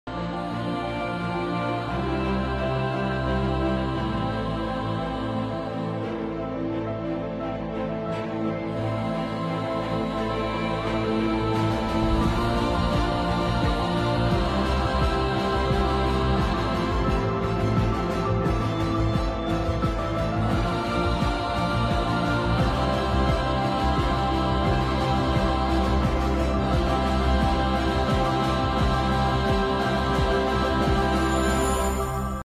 الإنسان المبدع يخلق الجمال من العدم.أعمال فنية رائعة من البسط 😍
الإنسان المبدع يخلق الجمال من العدم.
أعمال فنية رائعة من البسط 😍